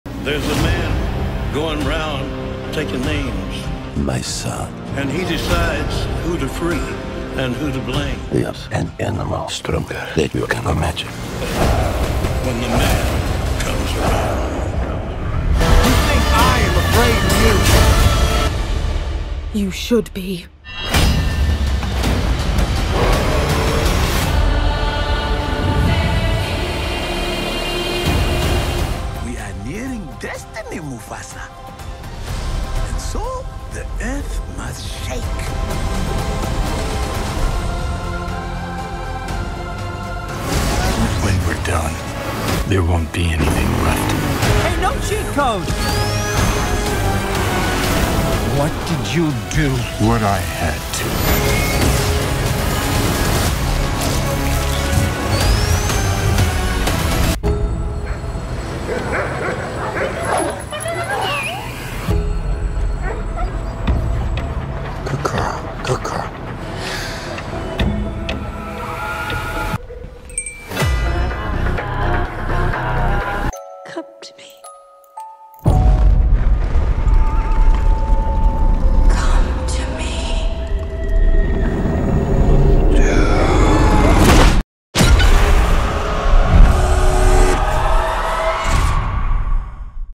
What's Playing at Landmark – December 2024
This December, experience adventure in Middle Earth, on the savannah, in the streets and in Transylvania with landmarkcinemas – Get your tickets